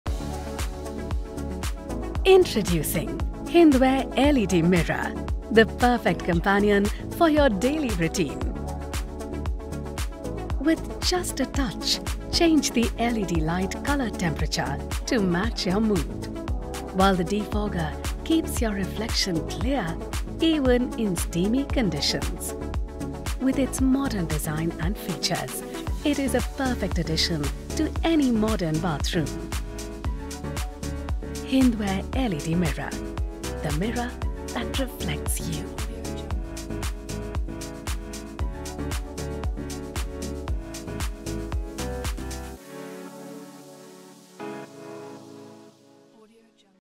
Introducing Smart LED Mirror: The Mirror that Reflects You | Hindware
Elevate your daily routine with the all-new Hindware Smart LED Mirror. Reflect your style and redefine your grooming experience with technology that lets you change the colour temperature with just one touch.
Discover the modern mirror that mirrors you in every way.